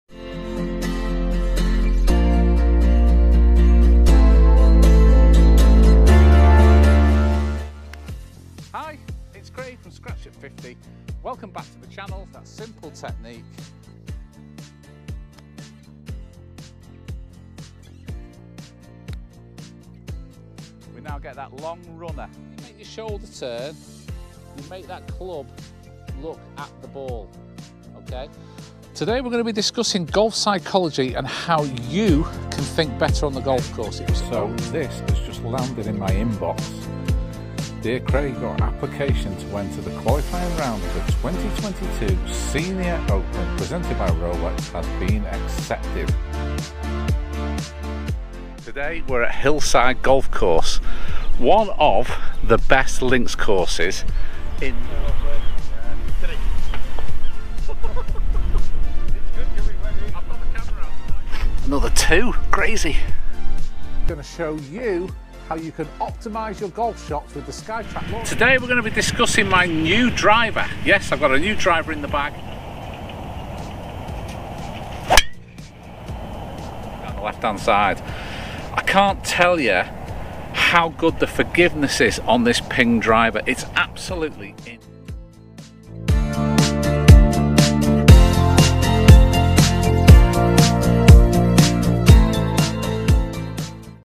Scratch At 50 Golf Channel Trailer
51 year old low handicap golfer flying the flag for the older guys n gals on You Tube.

This channel is a hobby for me so I don't post golf videos for the sake of posting golf videos.
I try to post golf videos that I hope you will find useful or enjoy watching.

- Golf tips
- Golf vlogs
- Golf Psychology Tips
- Equipment Reviews
- Senior Open qualifying series
- My club competition rounds in 2023

I have played off between +1 and 2 handicap for over 35 years and played as a full time amateur for 2 years in my youth.

Ive played in multiple English Amateur stroke play and match play championship's as well as the British Amateur .
Through this channel I will pass on what I have learned on that journey and have some fun a long the way.